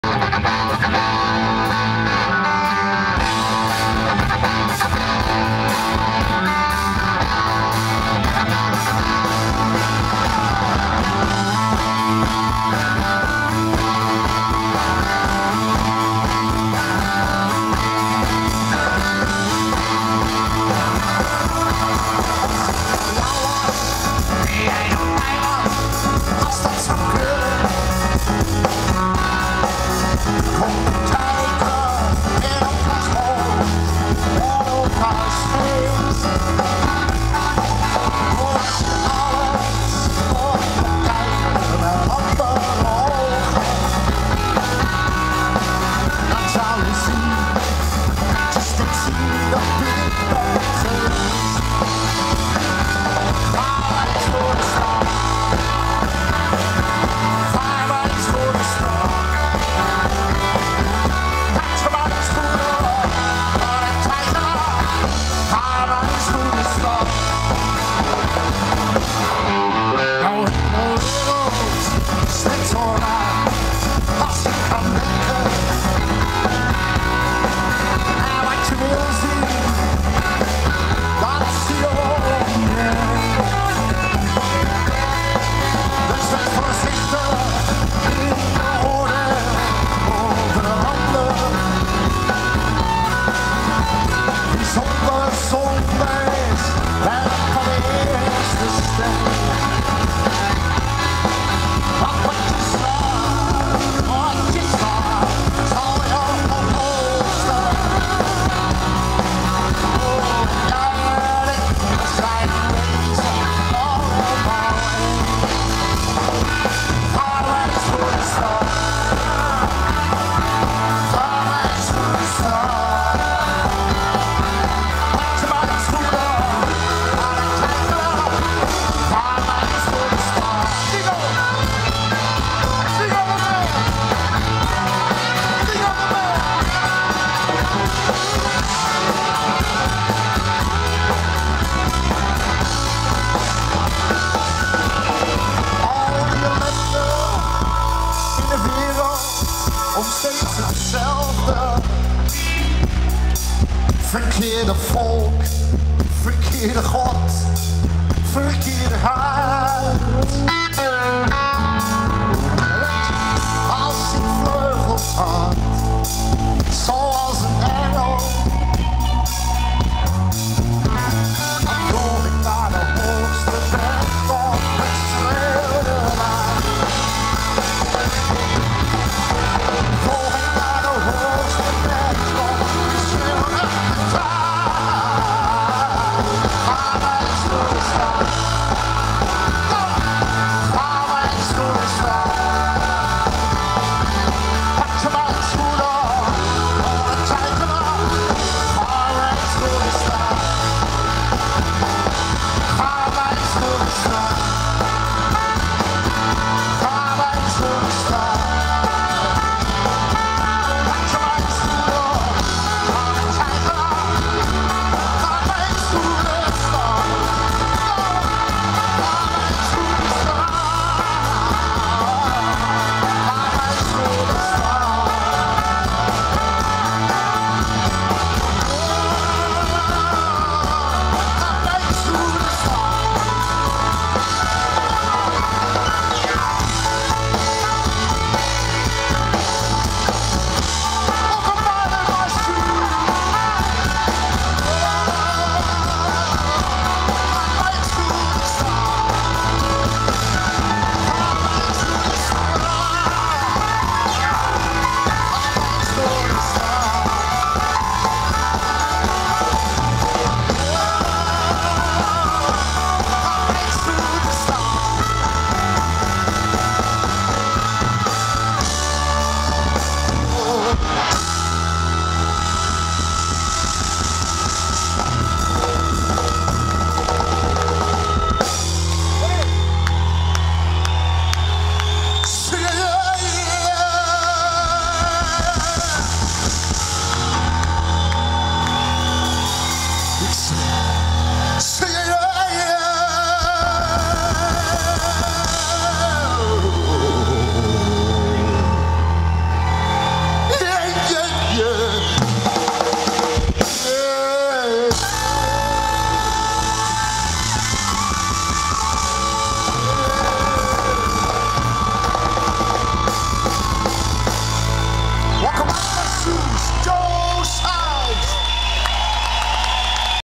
Indian Summer Festival 2010 - De Dijk
Indian Summer Festival 2010 - De Dijk with a raincoat on due to the (short) rainfall (the raincoats were provided for free to the audience)